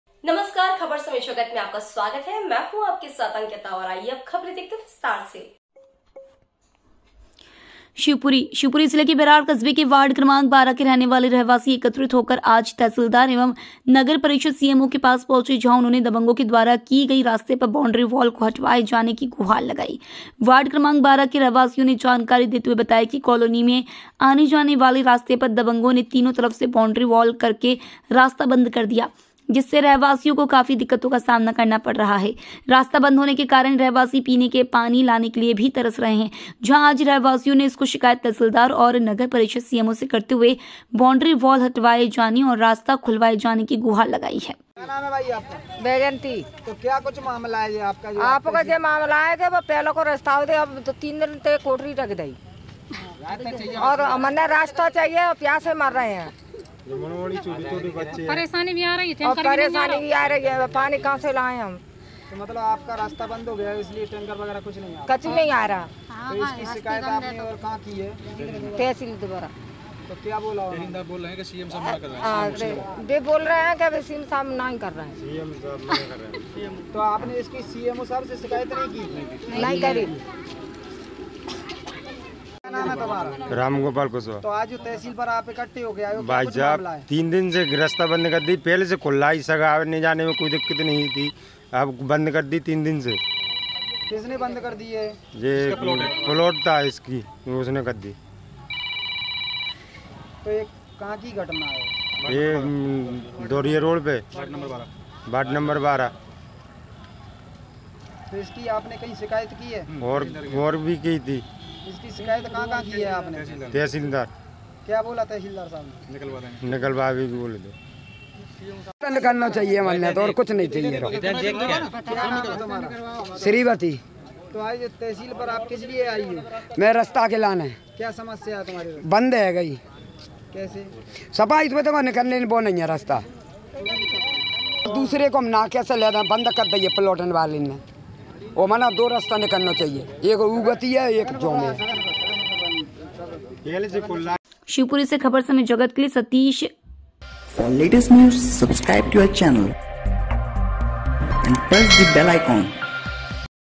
शिवपुरी | दबंगों ने रास्ते पर की बाउंड्री वार्डवासियों ने तहसीलदार और सीएमओ से की शिकायत | news
शिवपुरी से सतीश धाकड़ की रिपोर्ट